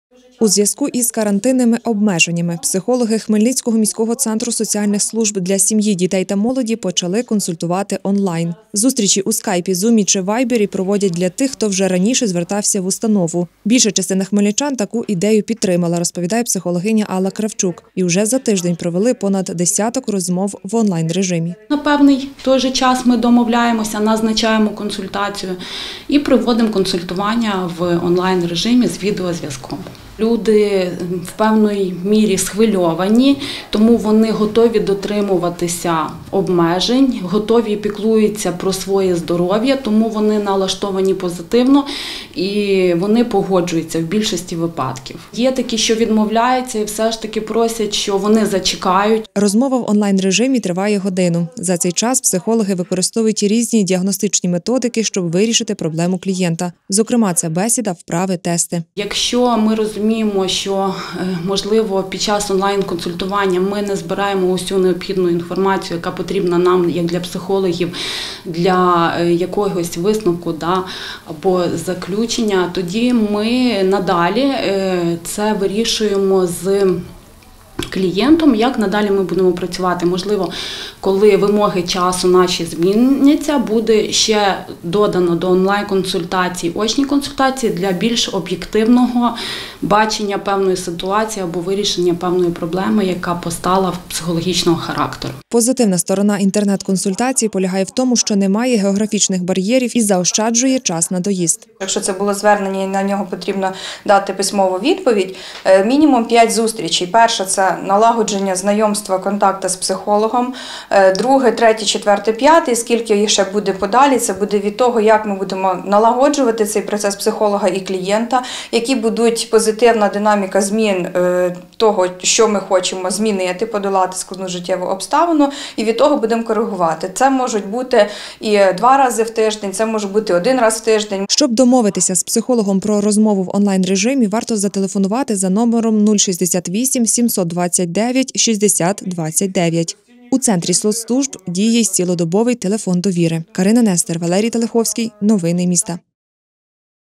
Консультація психолога в режимі онлайн
Проконсультують через відеозв’язок. Відтепер фахівці Хмельницького міського Центру соціальних служб для сім'ї, дітей та молоді розмовляють з клієнтами в онлайн-режимі. Таку практику у зв’язку із карантинними обмеженнями запровадили з початку цього тижня. Під час зустрічей психологи працюють з нескладними кризовими станами людини, допомагають вирішити конфліктні ситуації у сім’ї чи на роботі.